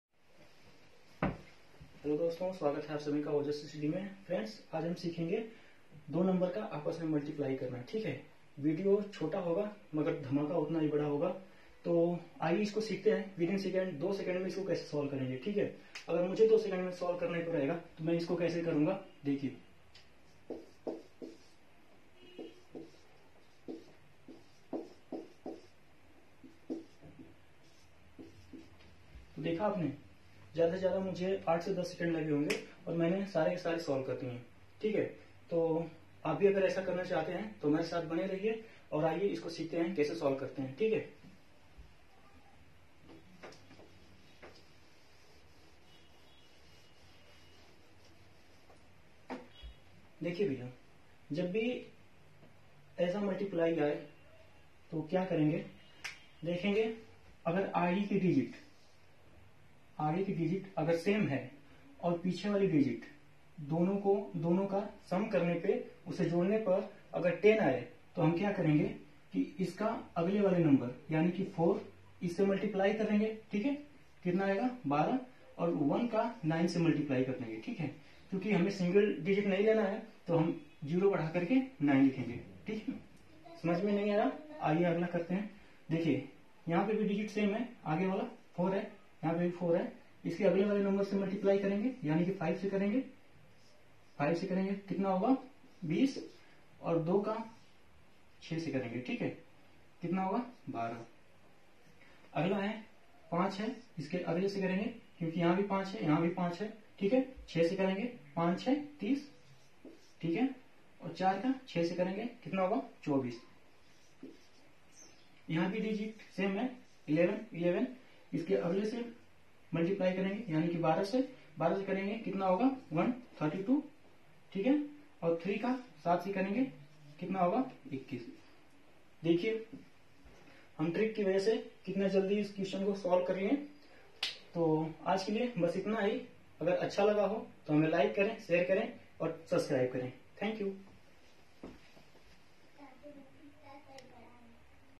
ojasstudy Multiplication in a second | मल्टिप्लिकेशन बस एक सेकंड में
नमस्कार दोस्तों!!
ये मल्टिप्लिकेशन की शॉर्टकट वीडियो है।
आशा करता हूँ आप को बहुत पसंद आएगा और इस वीडियो से आप को बहुत कुछ सिखने को मिलेगा। जो की आप के एग्जाम में एक नंबर का आना ही आना है। इस वीडियो को सभी परीक्षाओ को ध्यान में रख कर बनाया गया है जो की आप के आने वाले एग्जाम के लिए बहुत ही महत्वपूर्ण है।